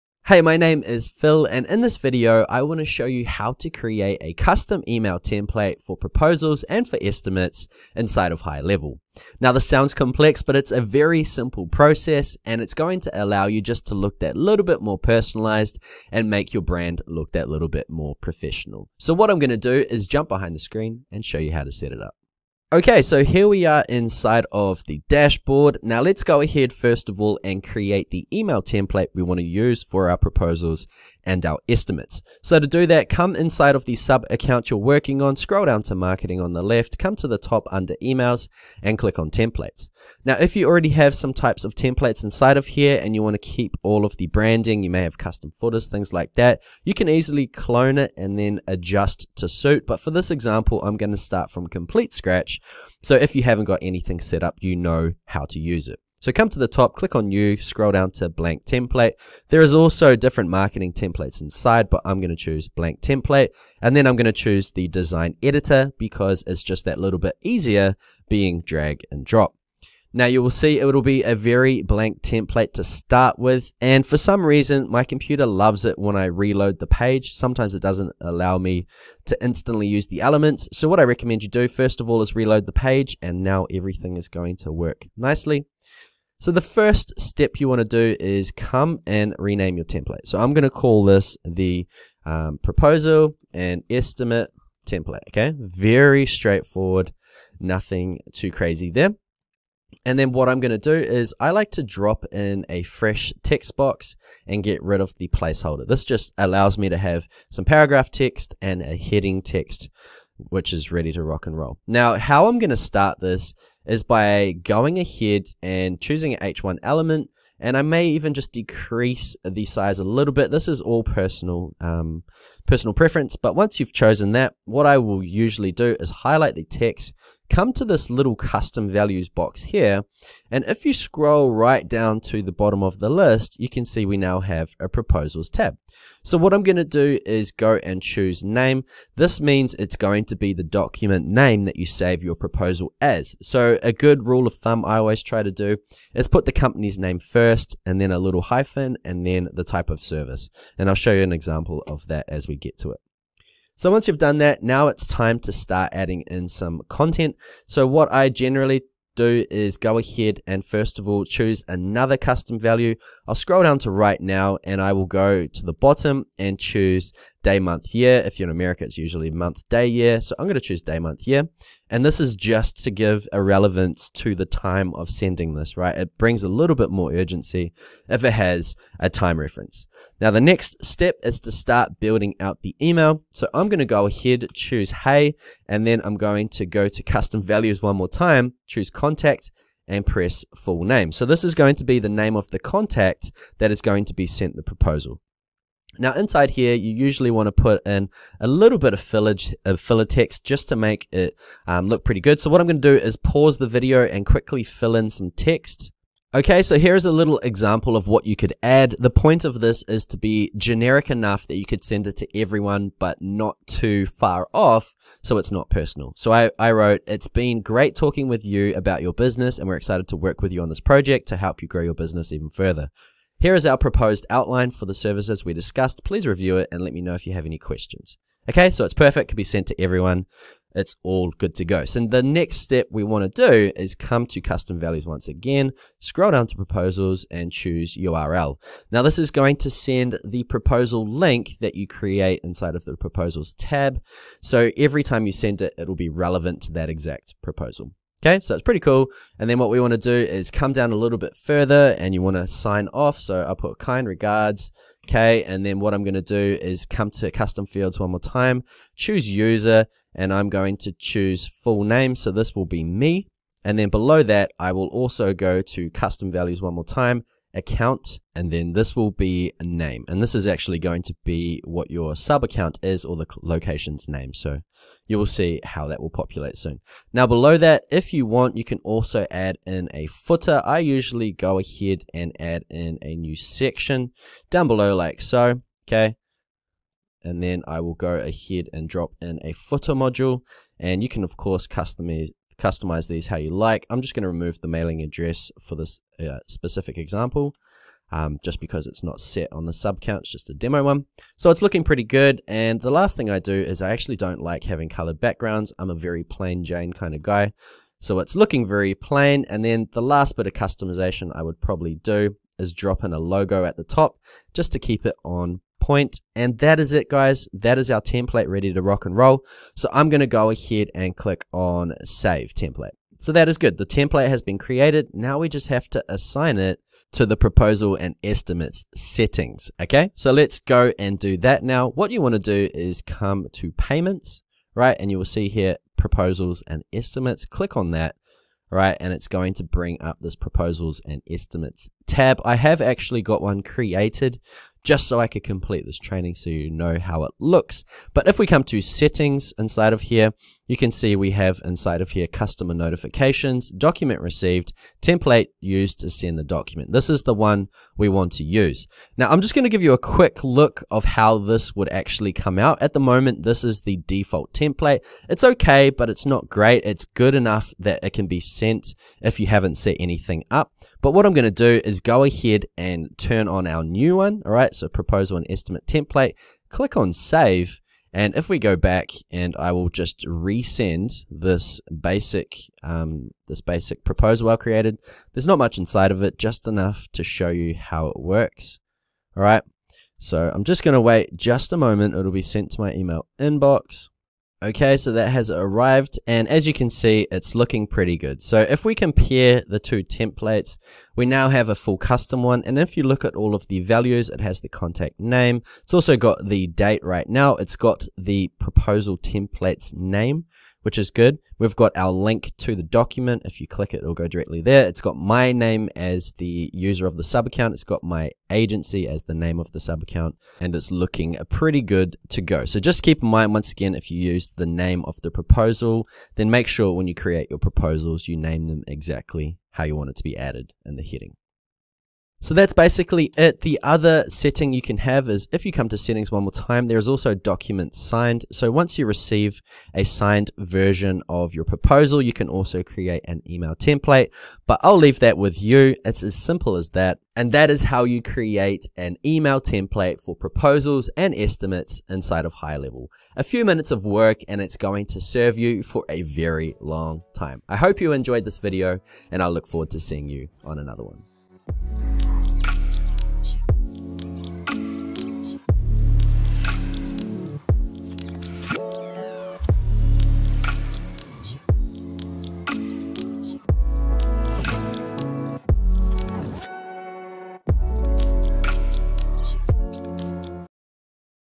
GoHighLevel Custom Proposal Email Templates ❇️ Super Easy!
Do you want to create your own custom proposal and estimate email templates for HighLevel? This video will walk you through step-by-step how to build your own custom proposal email templates for GoHighLevel.

0:00 GoHighLevel Custom Proposal Email Templates
0:23 How to tutorial
1:10 Proposal Email Template
5:50 Email Settings
6:35 Final Result